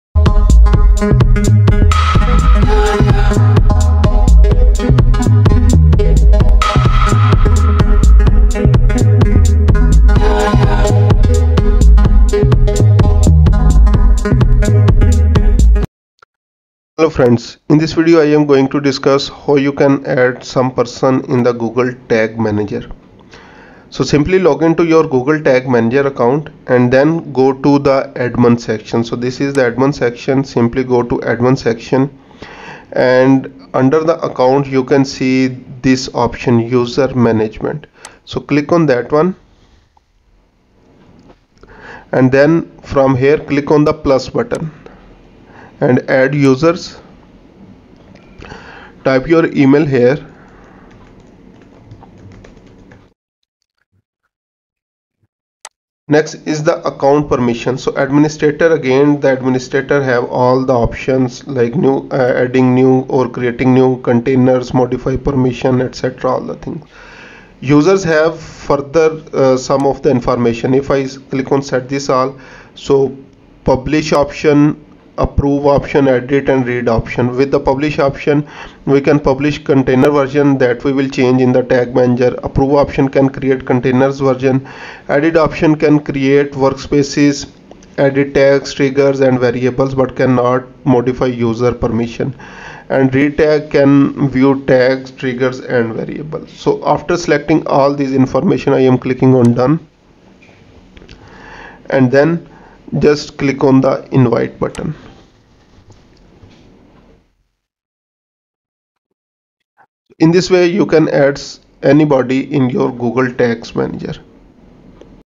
How to Get Access of Google Tag Manager
In this video, we are going to tell you about "How to Get Access of Google Tag Manager"

Can you access Google Tag Manager from Google Analytics?
First, after creating your free Google Tag Manager (GTM) account, you will want to link your existing Google Analytics Tracking ID by creating a New Tag in the Workspace. The new tag will track all of your website page views and provide you with all data reports available in Google Analytics.

Is Google Tag Manager the same as Google Analytics?
Google Analytics is the tracking tool, while Google Tag Manager is the mediator between your website and the tracking tool. In other words, Google Analytics collects, stores, and analyzes data. Google Tag Manager sends the data from your website to Google Analytics (or other tools) in the form of Tags.

Why should I use Google Tag Manager?
Increase your agility. Tag Manager gives you the ability to add and update your own tags for conversion tracking, site analytics, remarketing, and more. There are nearly endless ways to track activity across your sites and apps, and the intuitive design lets you change tags whenever you want.